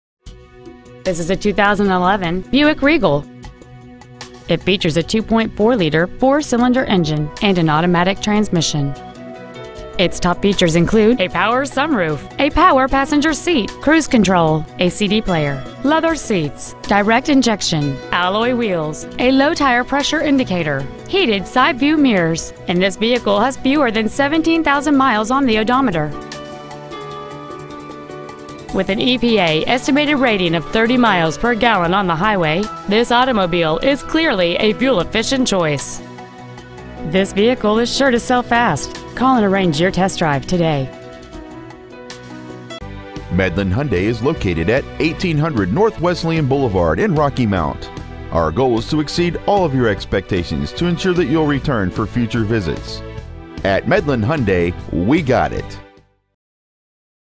2011 Buick Regal Rocky Mount NC
We are proud to present this 2011 Buick Regal .

 Year : 2011

 Make : Buick

 Model : Regal

 Engine : 2.4L 4 Cyl. Direct Injection

 Trans . : Automatic

 Exterior : Quicksilver Metallic

 Miles : 16,253

 Medlin Hyundai

 1800 N. Wesleyan Blvd

 Preowned 2011 Buick Regal Rocky Mount NC